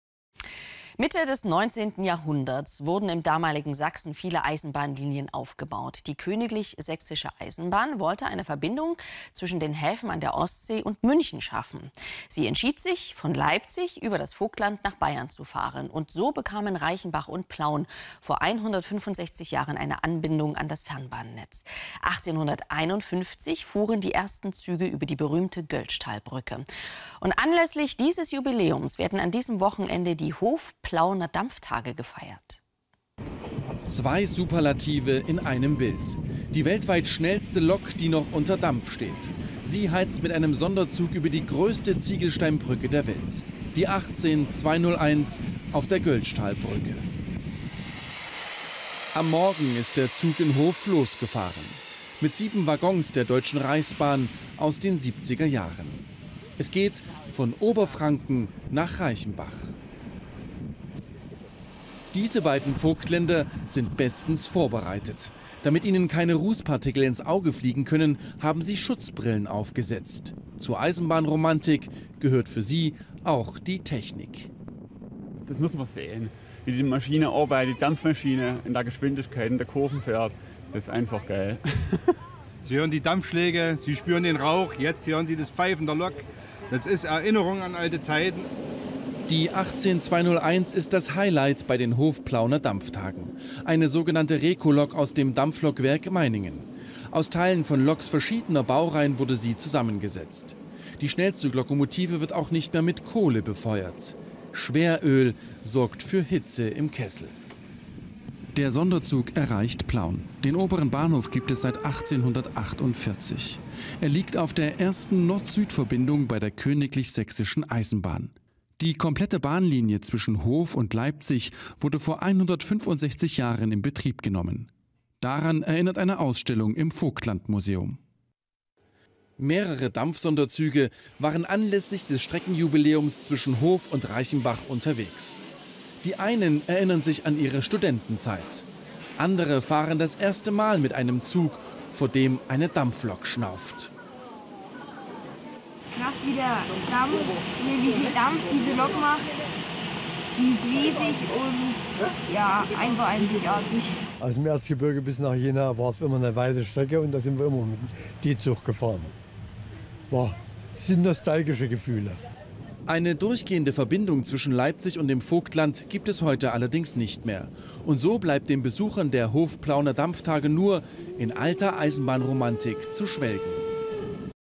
Nostalgiedampf
Nostalgie pur bei den Hof-Plauenern Dampftagen

1851 fuhren die ersten Züge über die Göltzschtalbrücke. Anlässlich dieses Jubliäums werden an diesem Wochenende die Hof-Plauener Dampftage gefeiert - mit vielen Dampfsonderzügen.

Quelle by MDR Sachsen